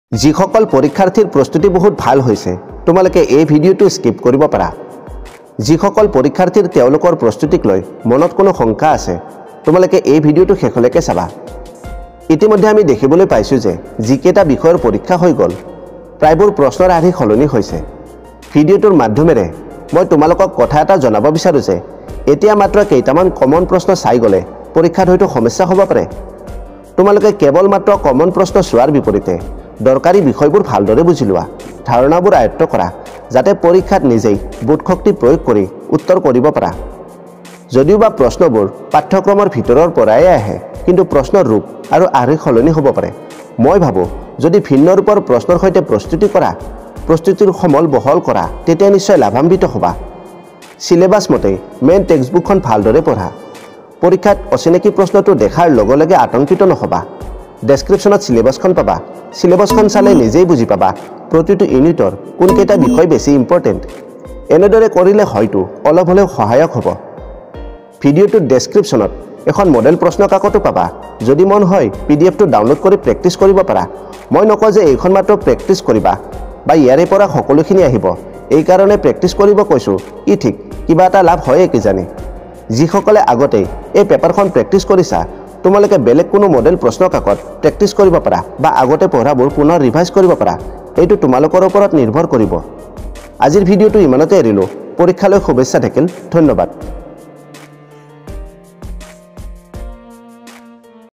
HS 2nd Year 2023 Chemistry Paper | HS 2nd Year final Exam 2023 Question Paper Chemistry | Class 12
In this video, I have tried a present a Model Question Paper for HS 2nd Year 2023 Chemistry Paper

Model Question papers are a good source for the students. It helps them in their studies and preparation for their exams as well. With the help of these papers they can have an idea about the questions patterns and they can acquire knowledge and get themselves prepared for the final exam.

Finally, students are adviced not to limit their sources for any preparation. Enlarge your knowledge & resources, this will help them to get a good result.

Hope you like this info provided by me. If you find this video informative & satisfied with this video,